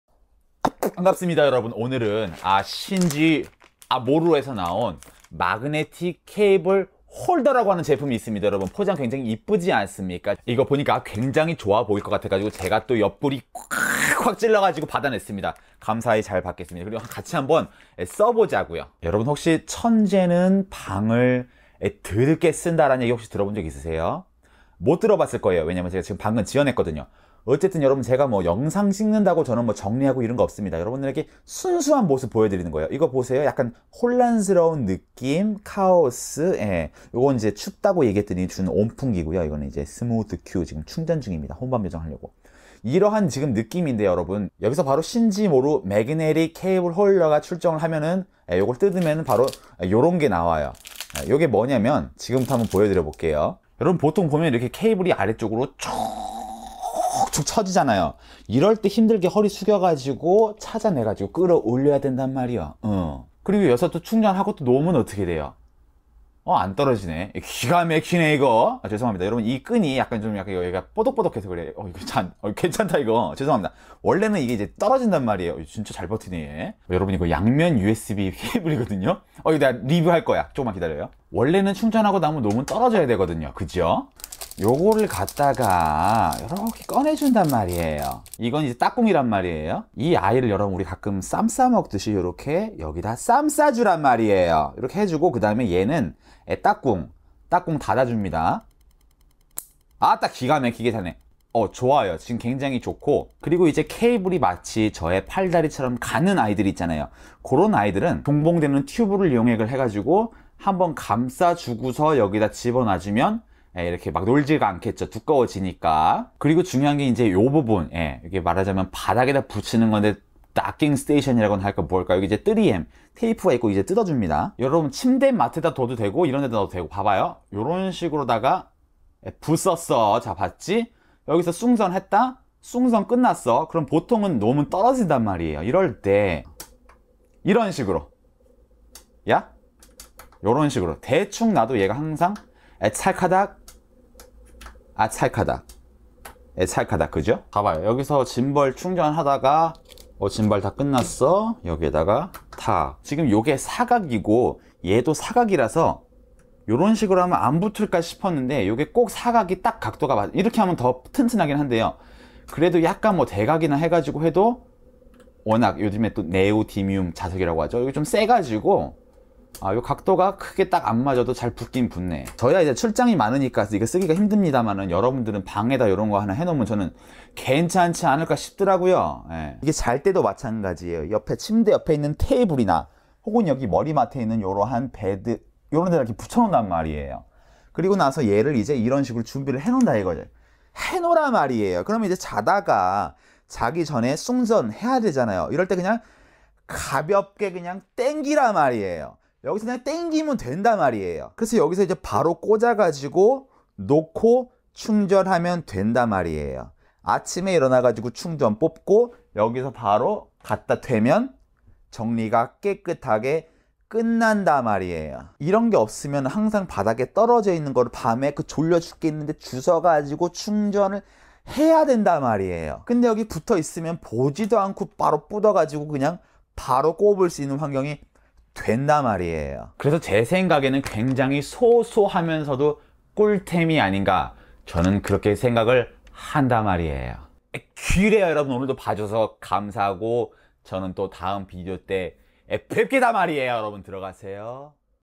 케이블 정리 꿀템!! 자석 케이블 홀더
신지모루에서 나온 마그네틱 케이블 홀더를 한번 사용해 보았어요
저는 출장이 많아서 못쓰겠지만 여러분들은 방이나 사무실에 세팅해놓으면
괜찮을 것 같습니다!!ㅎㅎ

Canon 80D

Zoom H1

Tascam DR-10L